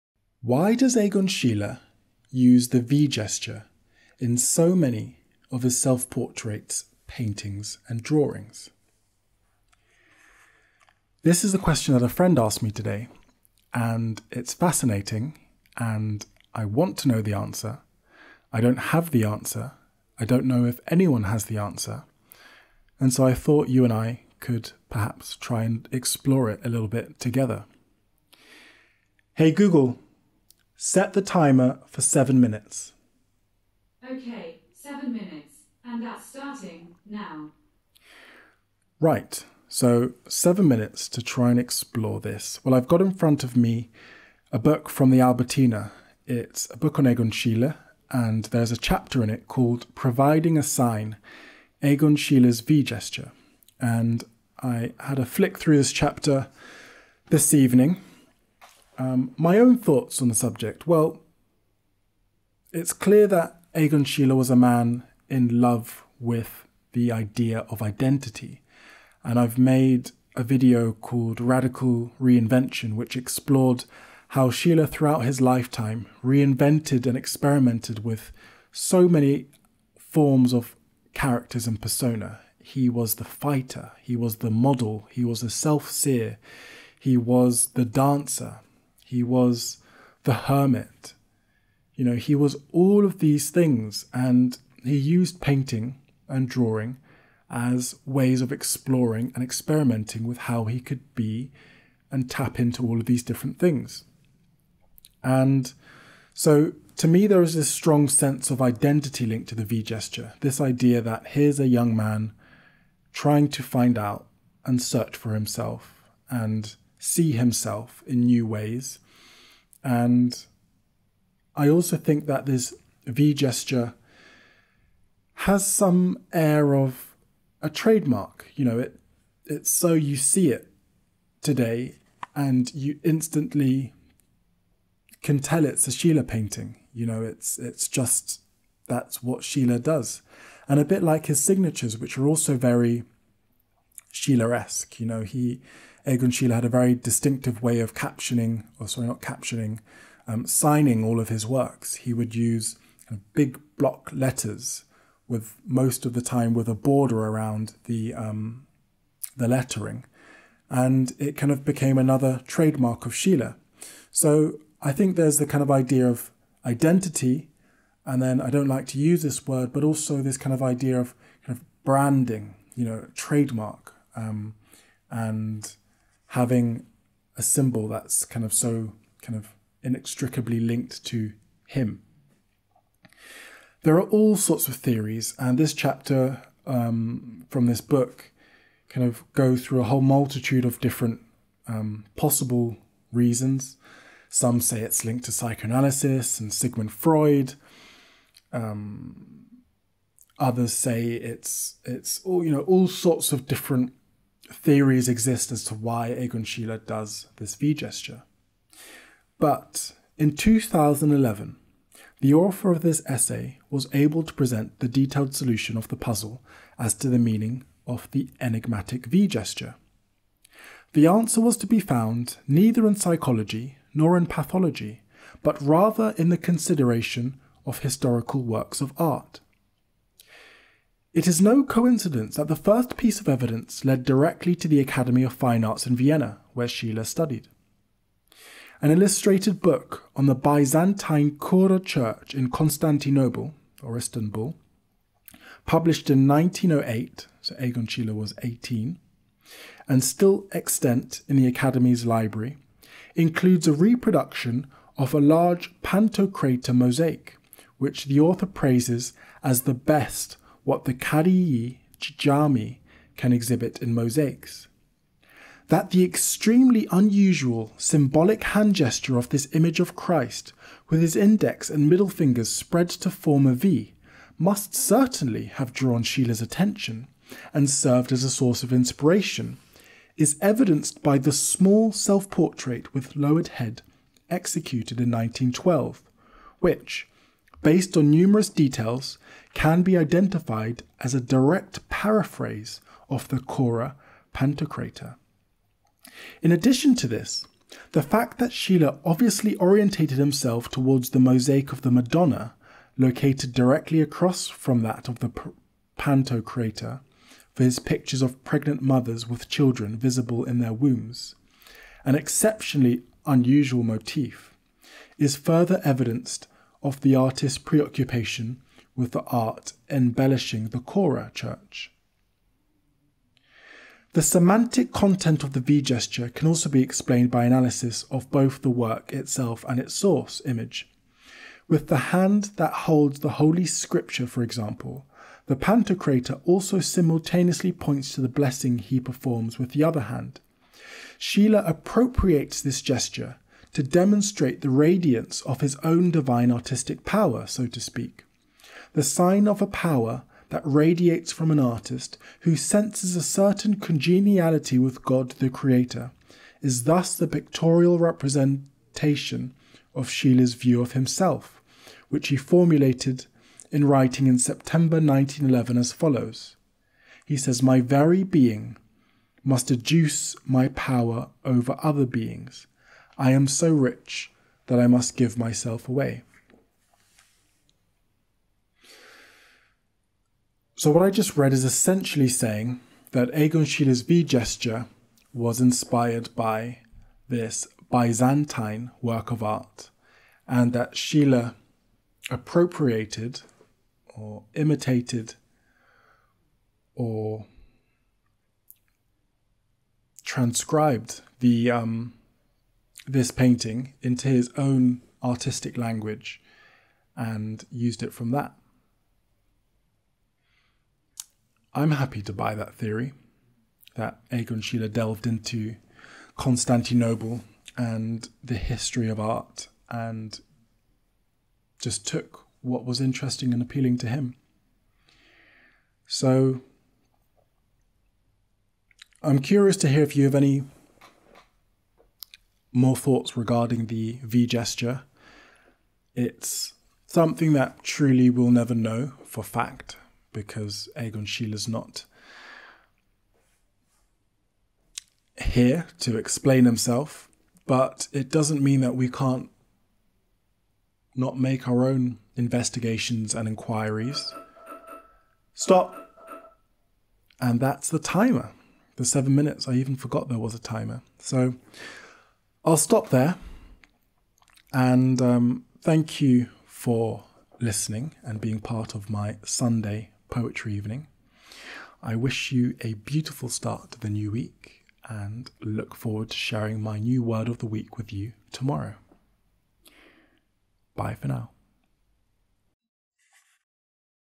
Egon Schiele's V-Gesture: On the Hanamichi Day 155
Why does Egon Schiele depict himself with these symbolic gestures? What does his infamous 'V-Gesture' mean and where does it come from? I try to answer these questions by giving my own perspective and sharing insights from the Albertina's 'Egon Schiele' book.

Catherine Suleiman
Chikako Hirayama 平山　千佳子
Joanna Lehnis
Mary Mc Caughey
Maria Leontine